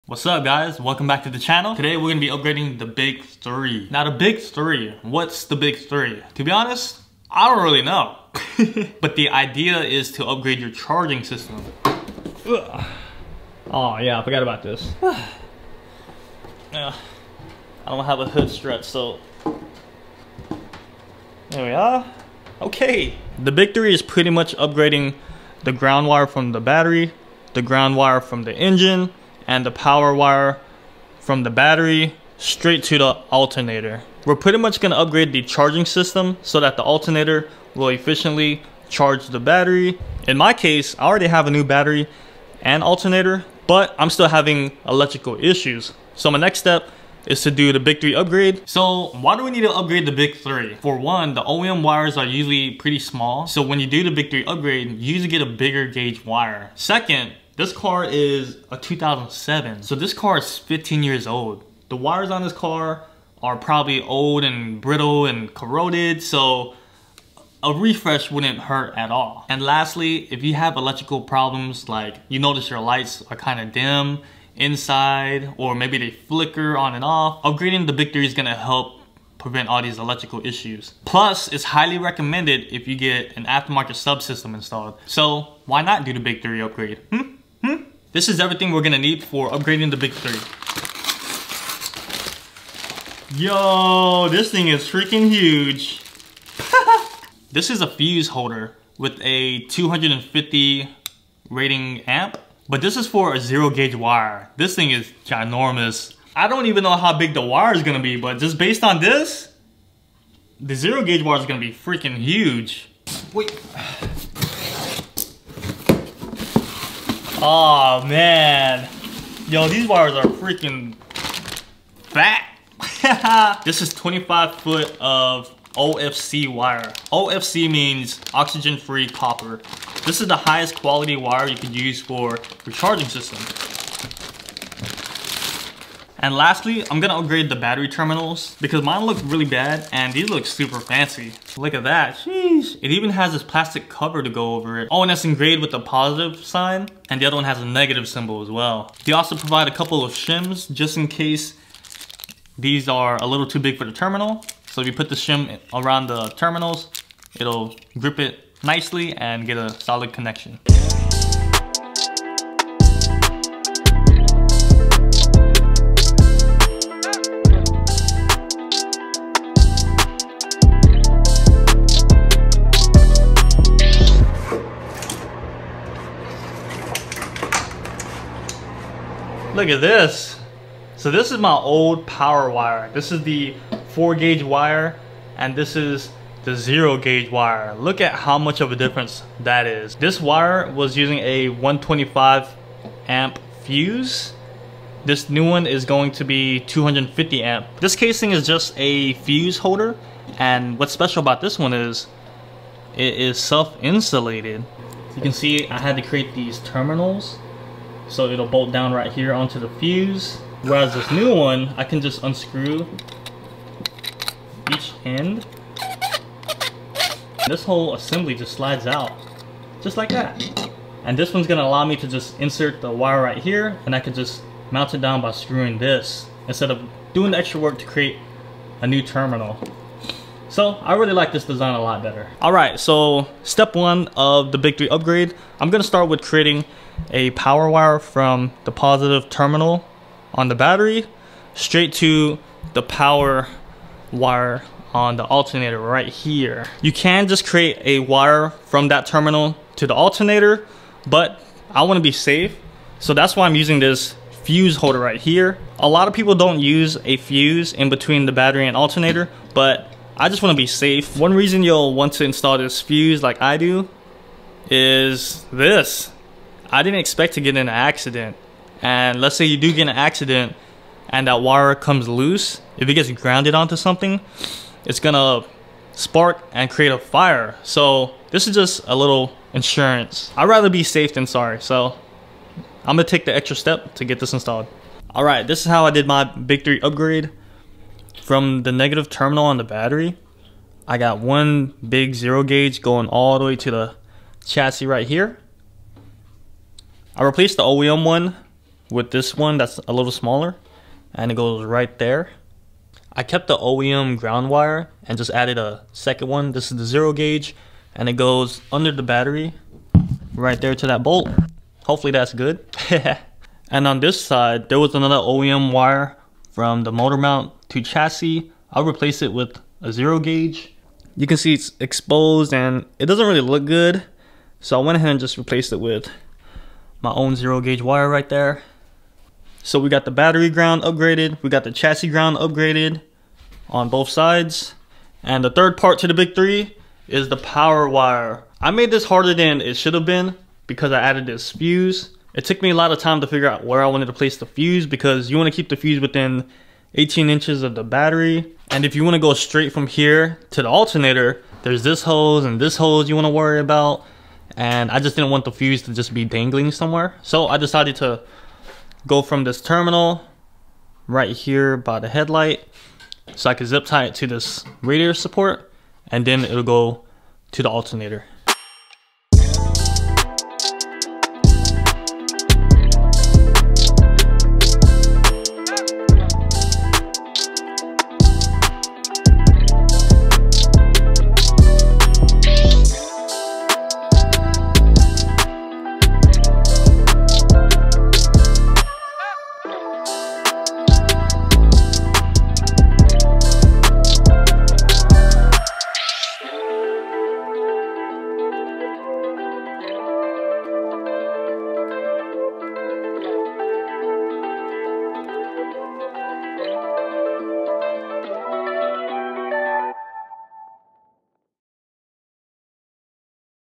3 Reasons to do the Big 3 Upgrade on your car - How to on 8th gen civic si
The Big 3 is referring to your car's charging system, in which the alternator recharges your battery in order for all of your electronics to function properly. I go over three main reasons why you should consider doing this upgrade on your car.

🛠 Big 3 Upgrade
► 0 Gauge OFC Wire
► 250A Fuse Holder
► Battery Terminal Connectors
► Anti-Corossion Washers
► Crimping Tool Kit